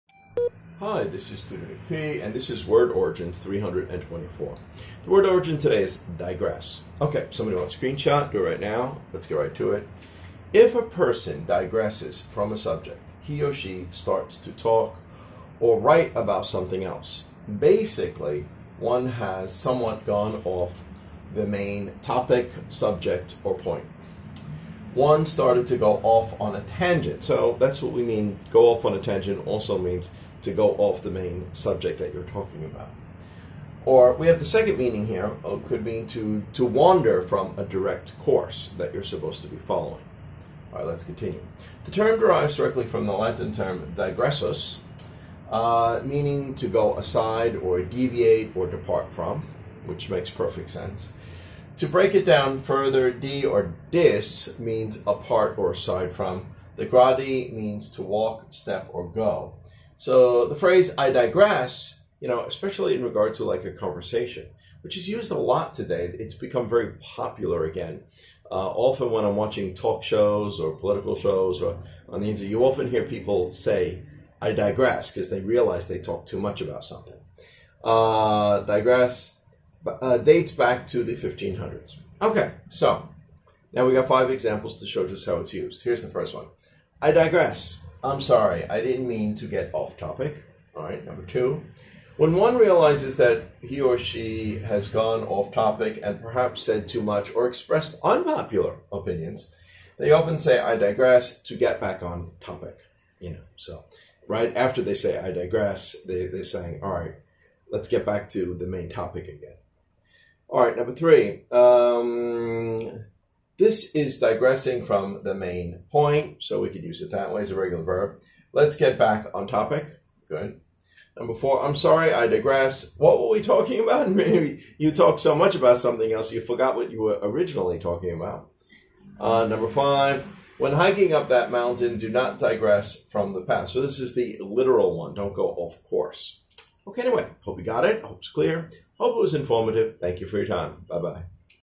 English Tutor Nick P Word Origins (324) Digress
In this video, we will cover this term, allude to its origin and give examples of this term.